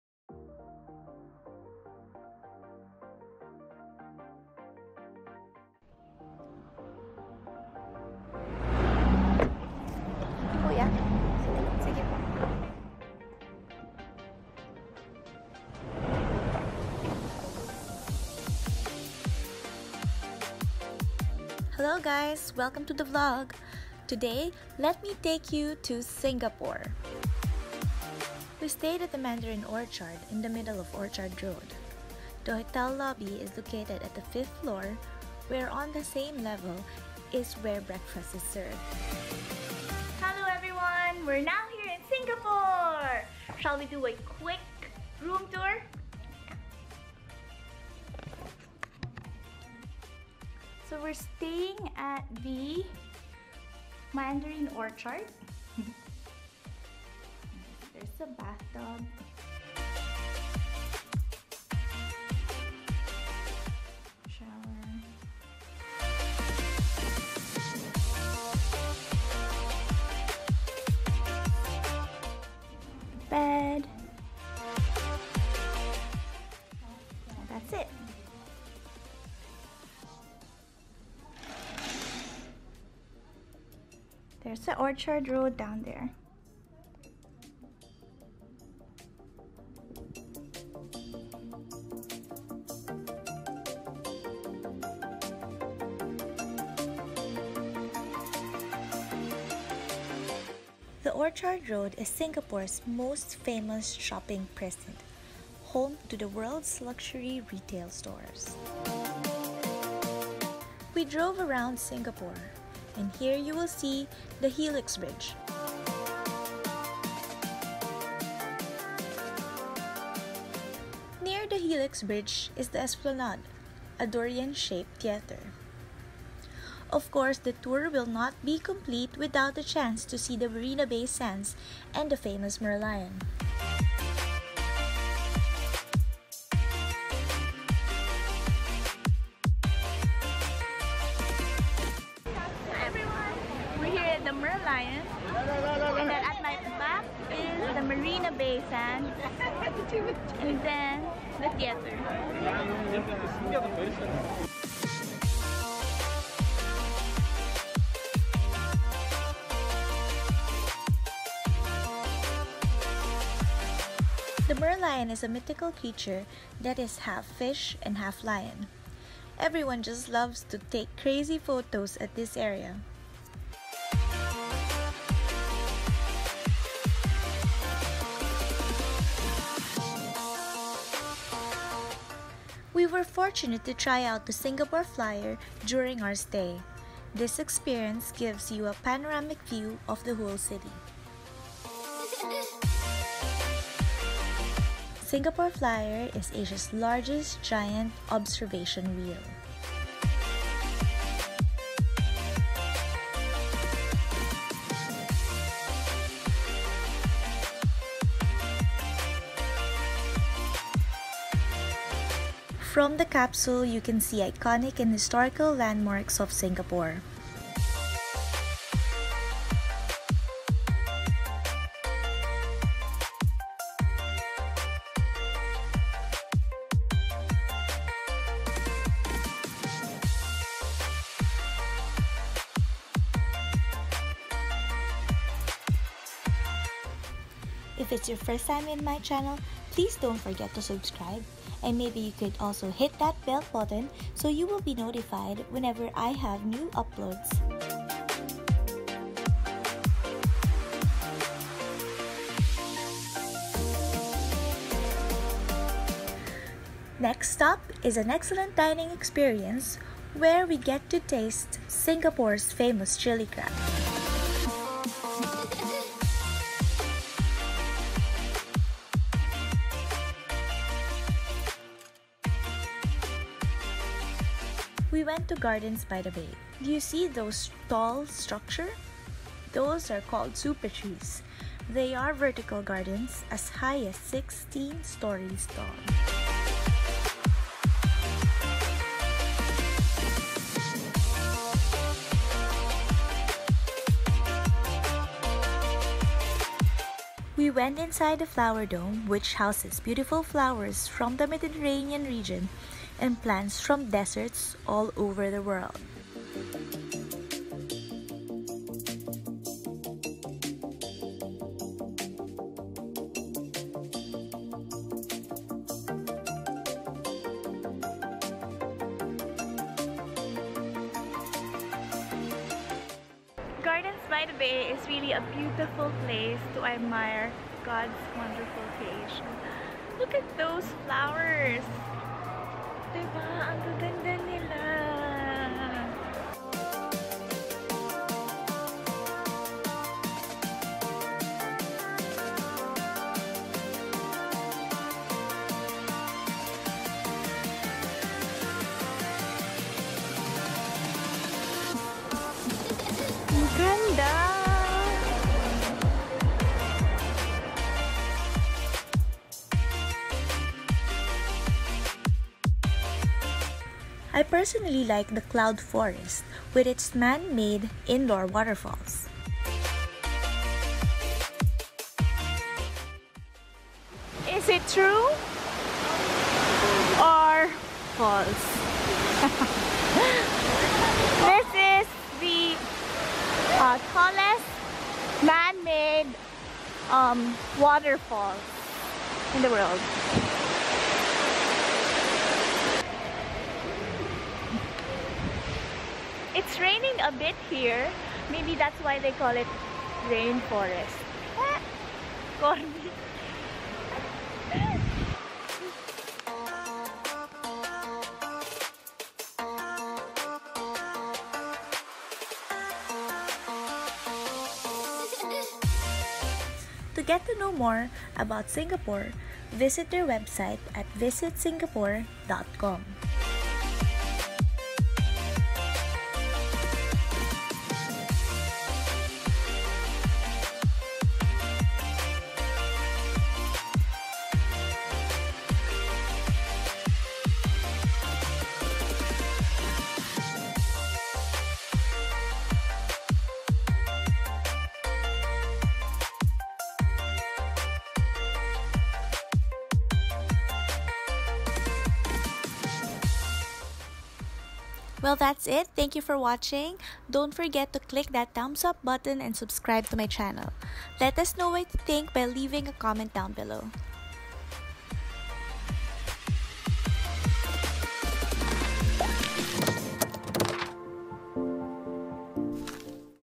Explore Singapore - City Tour, Singapore Flyer and the Gardens By The Bay [Oct 2018]
A quick trip to Singapore where we stayed at the Mandarin Orchard. Had a city tour - drive by of some of the iconic spots of Singapore. The Helix Bridge, Esplanade, Marina Bay Sands, Merlion, a ride at the Singapore Flyer and the Gardens By The Bay.